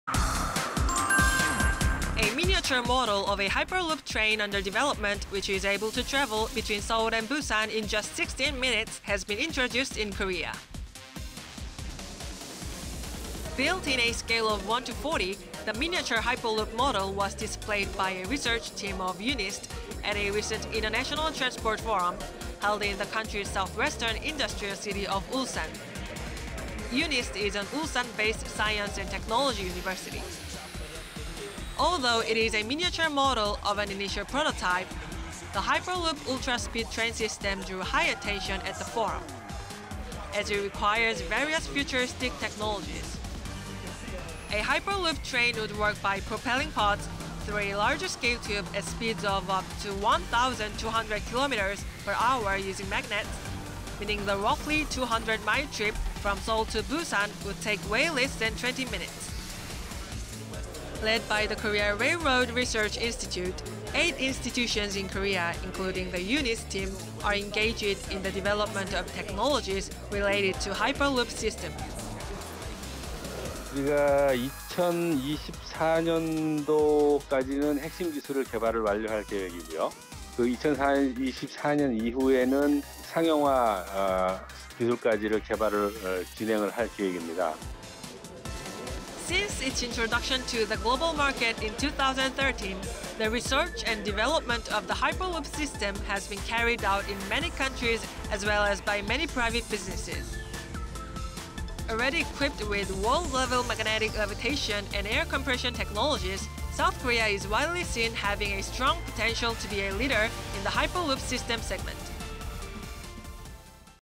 Miniature Hyperloop Model Introduced in Korea / YTN KOREAN
A miniature model of a hyperloop train under development, which is able to travel between Seoul and Busan in just 16 minutes, has been introduced in Korea.

Built in a scale of 1:40 (one to forty), the miniature hyperloop model was displayed by a research team of UNIST at a recent international transport forum held in the country's southwestern industrial city of Ulsan. UNIST is an Ulsan-based science and technology university.

Although it is a miniature model of an initial prototype, the hyperloop ultra-speed train system drew high attention at the forum as it requires various futuristic technologies.

A hyperloop train would work by propelling pods through a large-scale tube at speeds of up to 1,200 kilometers per hour using magnets, meaning the roughly 200 mile trip from Seoul to Busan would take way less than 20 minutes.

Led by the Korea Railroad Research Institute, eight institutions in Korea, including the UNIST team, are engaged in the development of technologies related to the hyperloop system.

[Dr. Lee Gwan-Seok, researcher, the Korea Railroad Research Institute]
"We plan to complete the development of core technologies (for the hyperloop system) by 2024 and then continue to develop commercial technologies."

Since its introduction to the global market in 2013, the research and development of the hyperloop system has been carried out in many countries as well as by many private businesses.

Already equipped with world-level magnetic levitation and air compression technologies, South Korea is widely seen having a strong potential to be a leader in the hyperloop system segment.